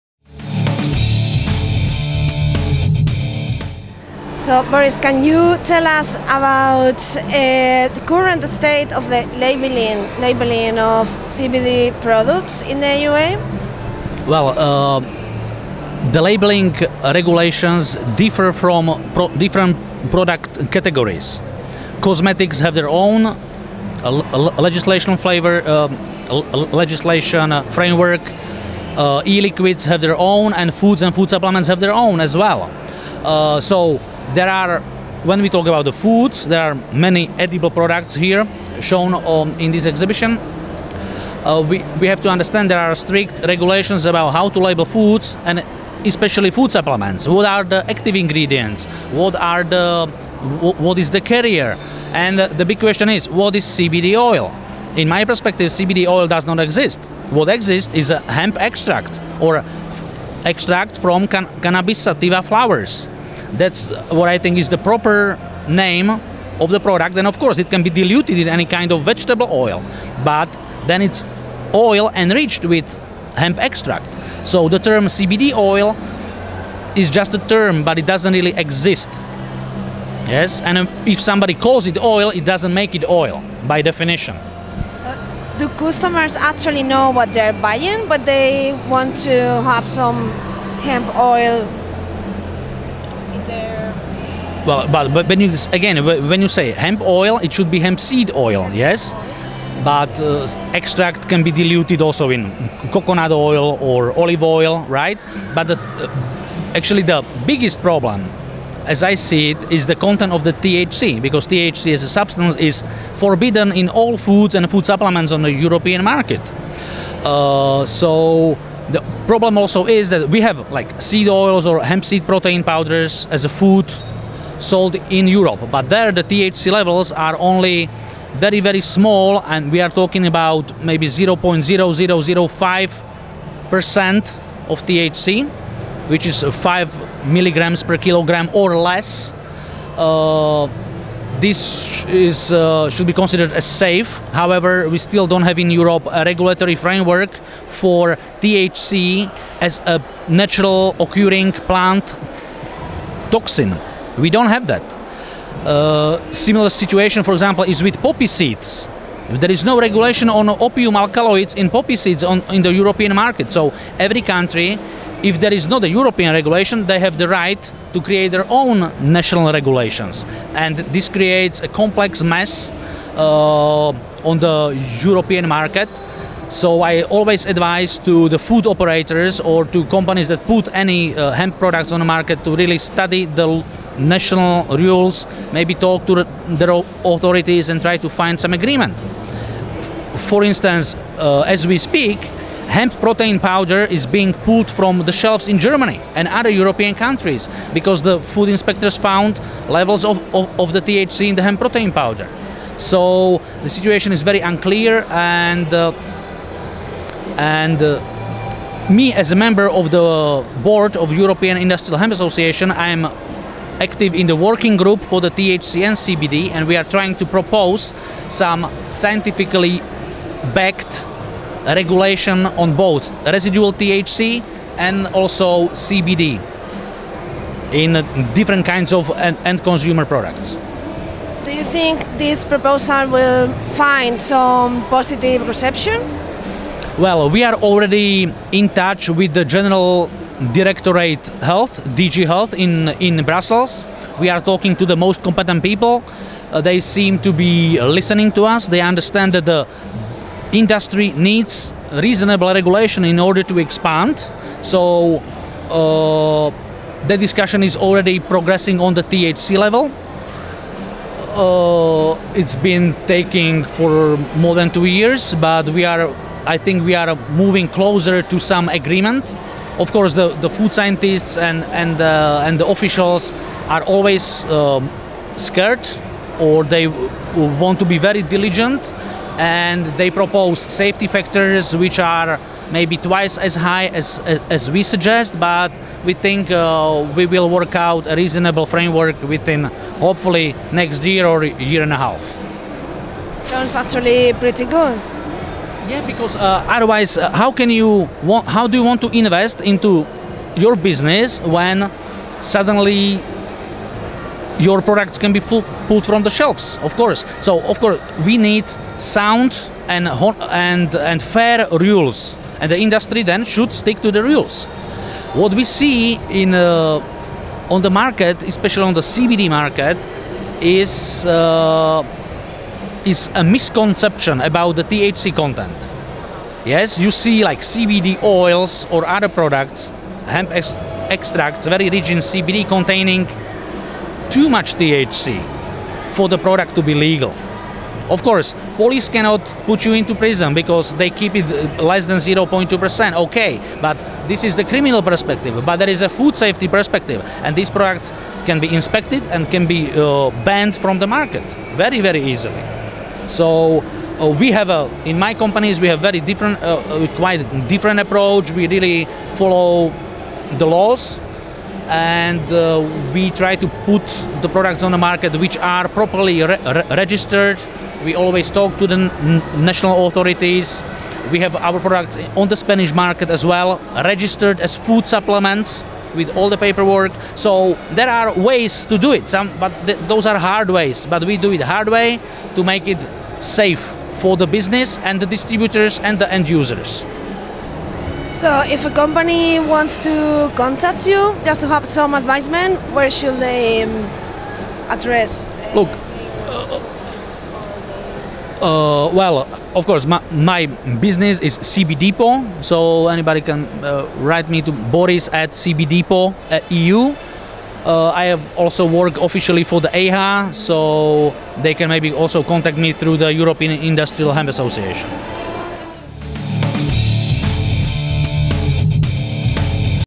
Interview Boris Baňas
Boris Baňas, Cannabis Activist and founder of CBDepot. Cultiva Hanfmesse, Vienna.